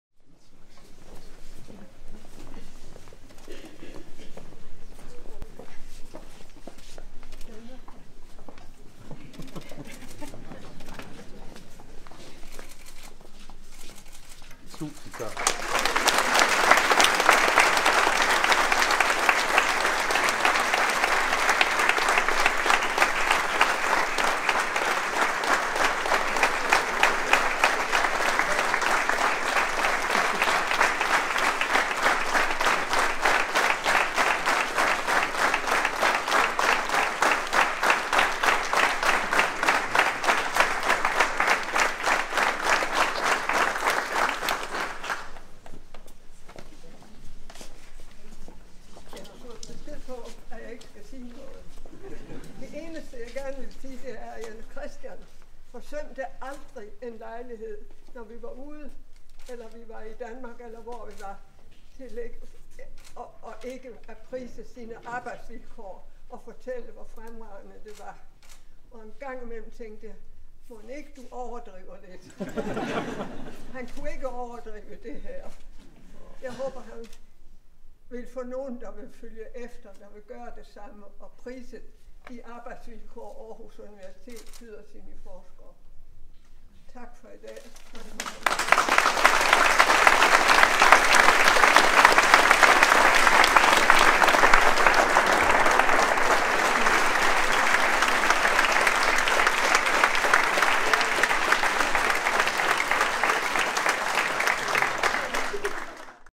Ellen Margrethe Skou klipper snoren til Skou-bygningen 8. oktober 2018
Jens Chr. Skous hustru, Ellen Margrethe Skou, klippede snoren til AU's nye biomedicinske forskningsbygning den 8. oktober 2018.